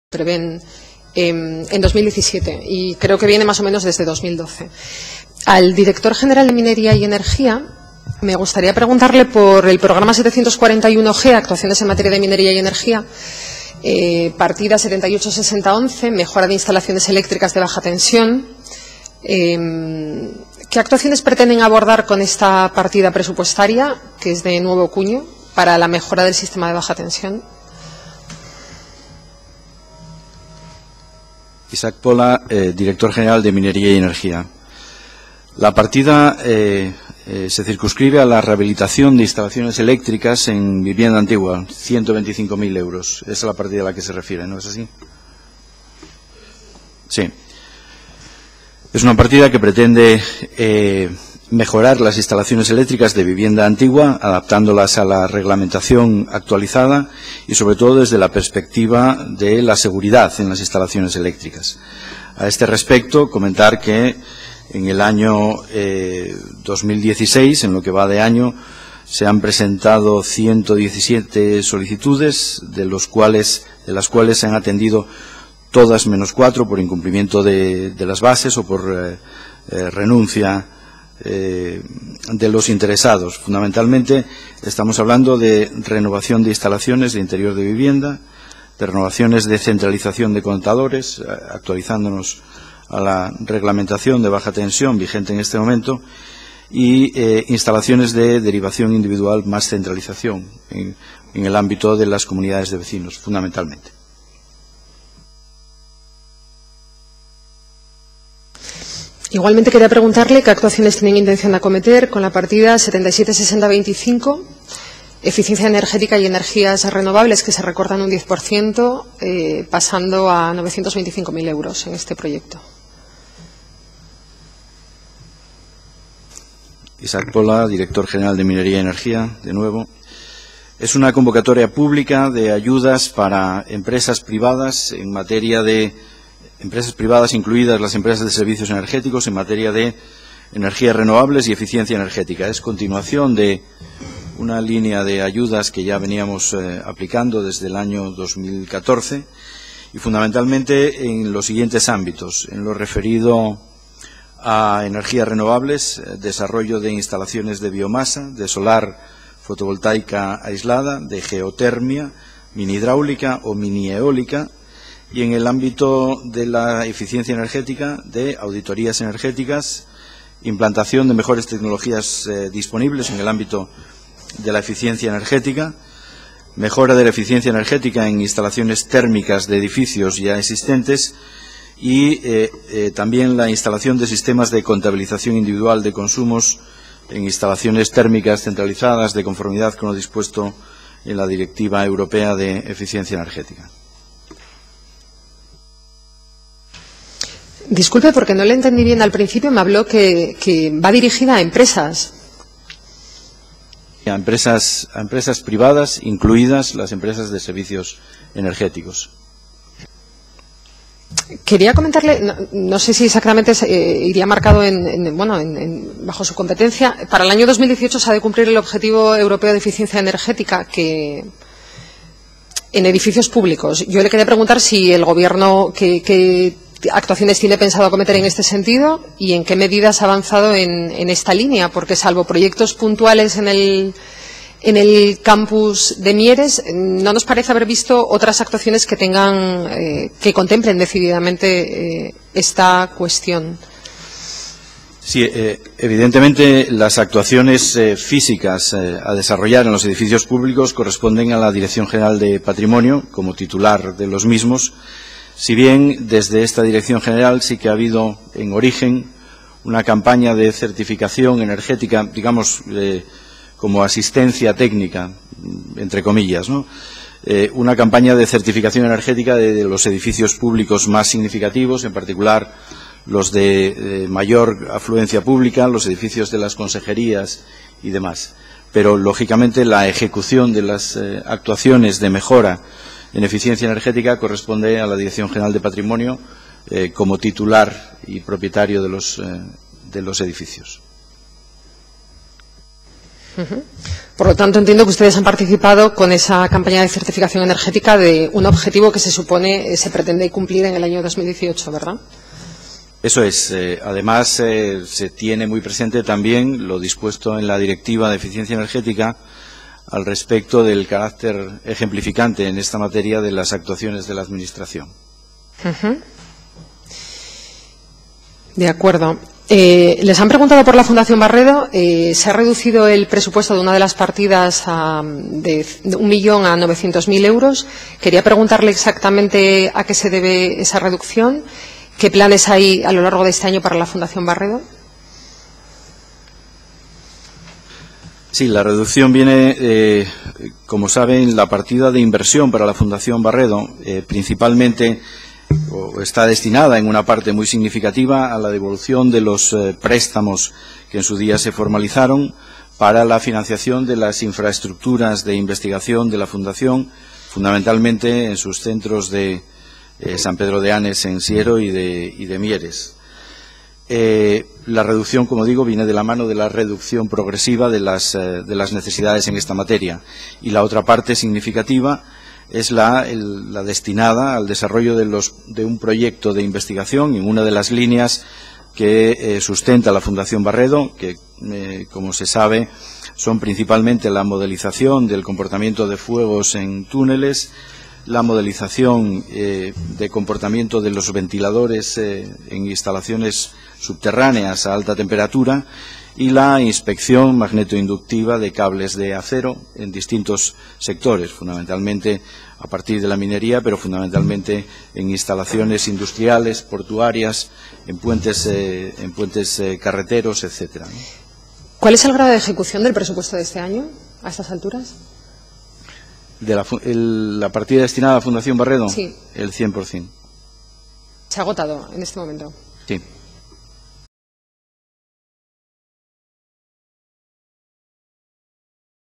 ¿Qué actuaciones pretenden abordar con la partida de la mejora del sistema de baja tensión?
Comparecencias Informativas Presupuestos 2017
Comisión de Hacienda y Sector Público
CONSEJERÍA DE EMPLEO, INDUSTRIA Y TURISMO

Director General de Minería y Energía: Isaac Pola Alonso